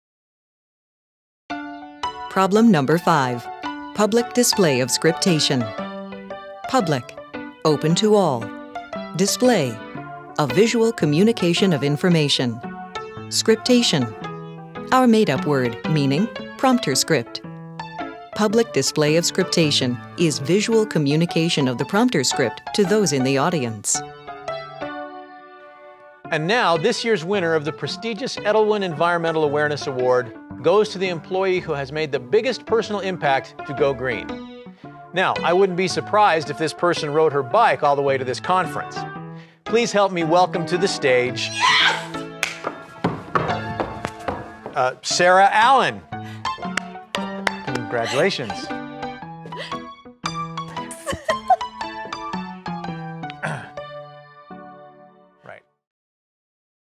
Jumbo Bright - Public Display of Scriptation
In public speaking, nothing is more embarrassing than the audience reading along as you deliver prepared remarks. This is typical with standard projection screen teleprompters, plasma displays on stands, and even floor monitors. Whether you're announcing a new product, presenting an award, or telling a joke, the audience should never beat you to the punch  or the punch line.